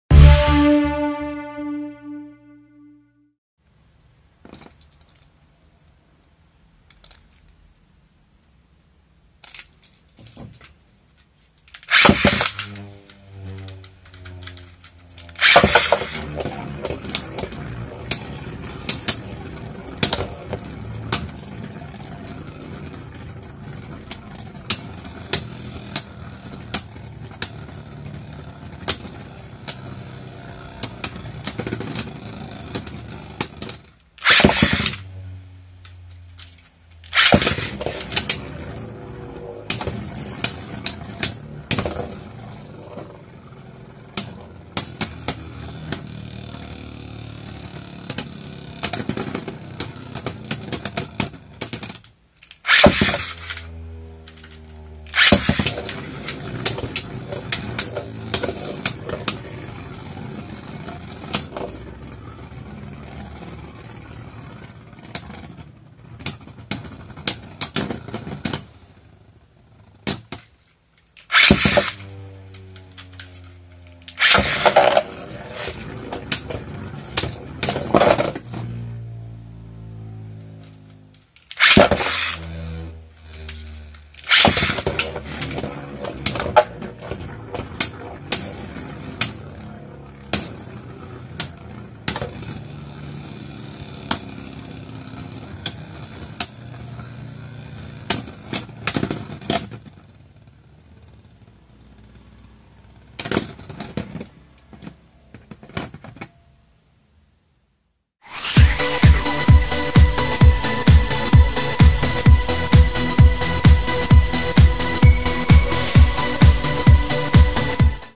Yugen Yegdrion .G.S vs Omni Odax .T.X - [Beyblade Burst Hasbro] - ベイブレードバースト
Testando um novo tipo de lançamento e um novo formato de vídeo! :D

Sabem, não está fácil encontrar músicas legais para colocar nos vídeos. Além disso, estou com muito pouco tempo para ficar procurando músicas. Eu também não gosto de ficar repetindo a mesma trilha por muitos vídeos seguidos, considerando que eu publico pelo menos uma vez ao dia. Então deixei sem mesmo. Vamos ver o que acontece. xD